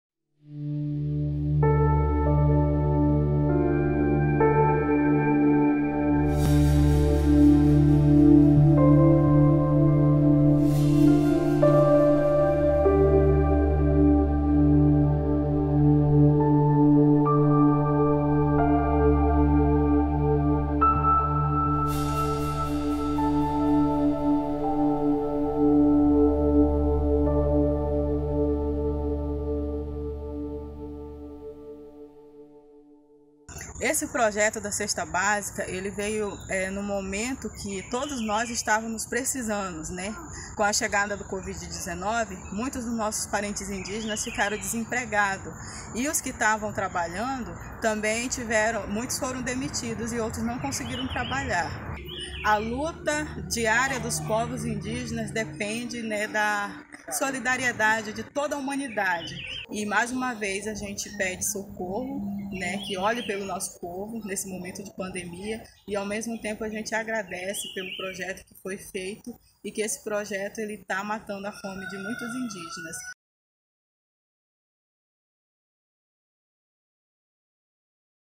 Alemanha apoia as comunidades indígenas no Brasil
A Alemanha apoia as comunidades indígenas no Brasil e ajuda centenas de famílias indígenas e ribeirinhas especialmente durante este período difícil da pandemia. Juntos somos mais fortes!